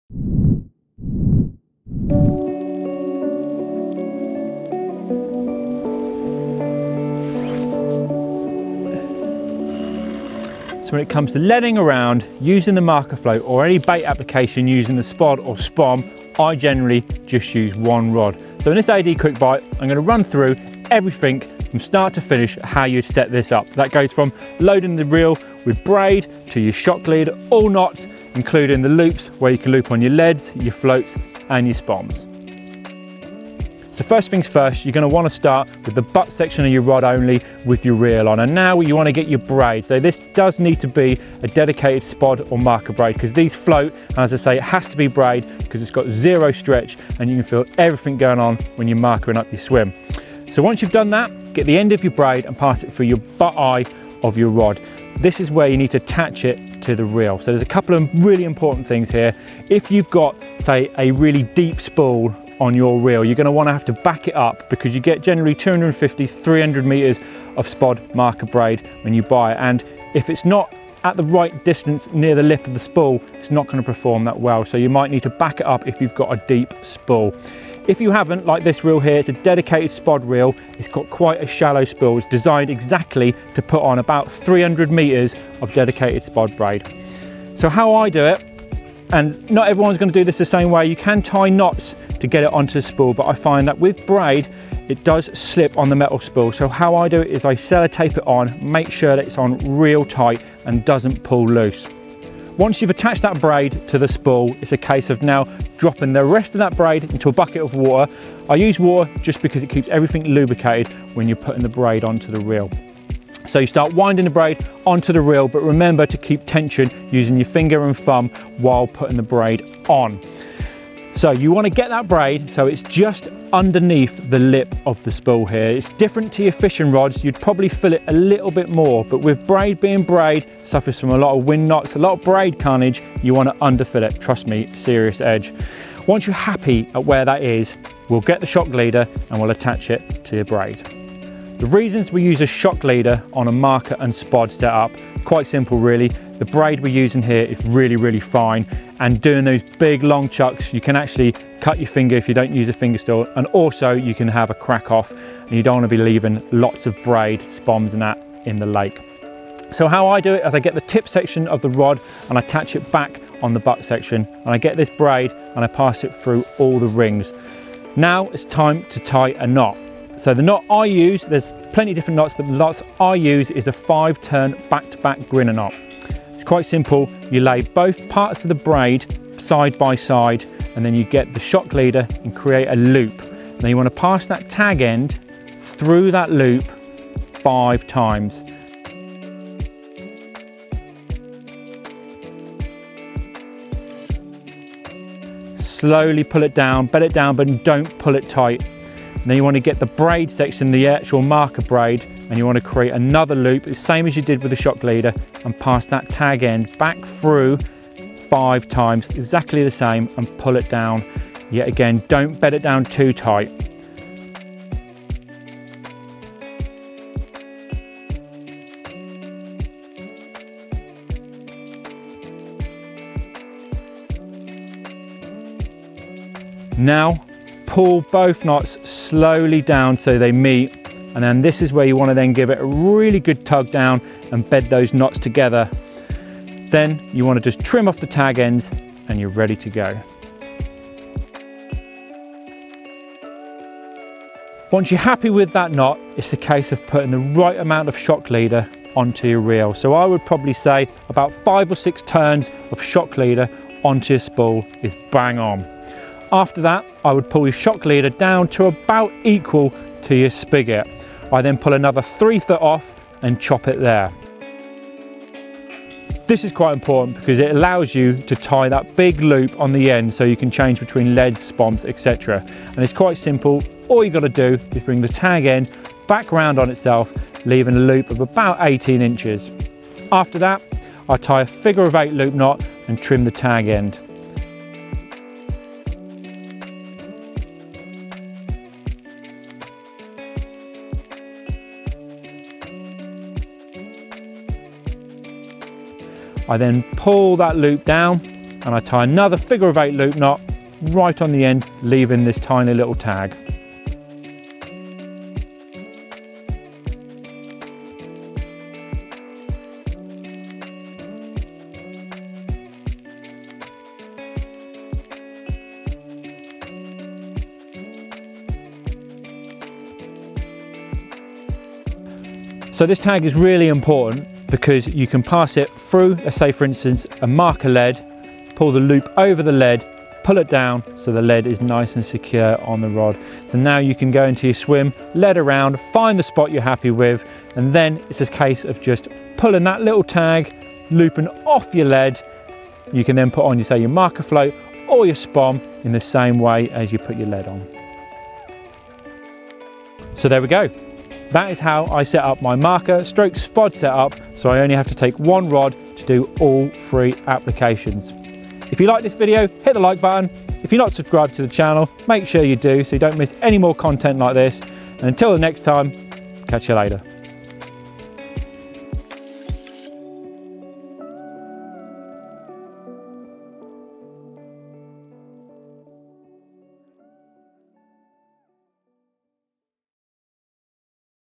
How To Set Up Your Spod/Marker Rod – Carp Fishing Quickbite
If it’s leading about, using a marker float or spombing out bait, with just one rod, you can do it all!
In this AD Quickbite, Team AD member Chris Ansell shows you how easy it is to use one rod for all three disciplines. Chris shows you the correct way to spool the reel, tie on a shocker leader and the all-important loop knot so that you can change between methods.